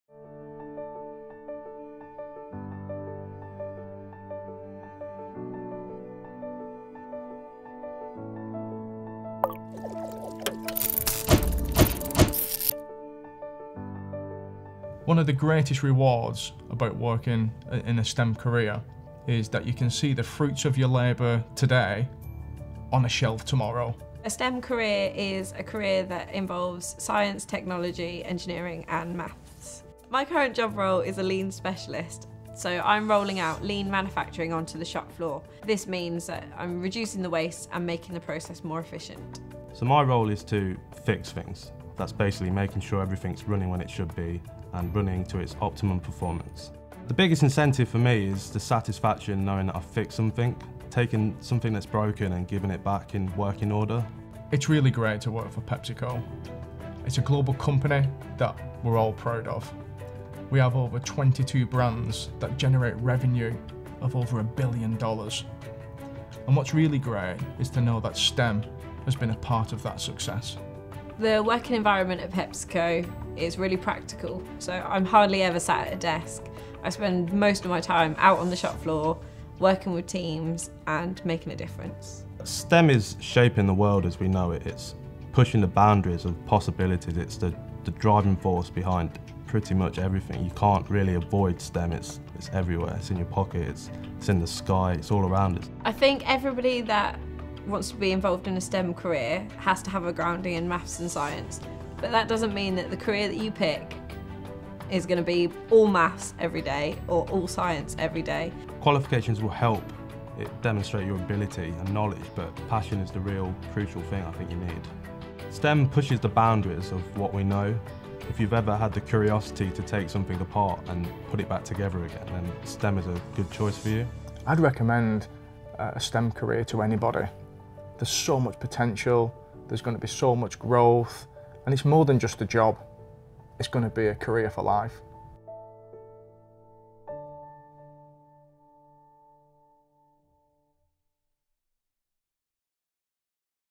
STEM skills and careers
PepsiCo explains what a STEM career is, and why we need people skilled in science, technology, engineering and maths to develop a wide range of products, and to drive innovation in the food and drink industry.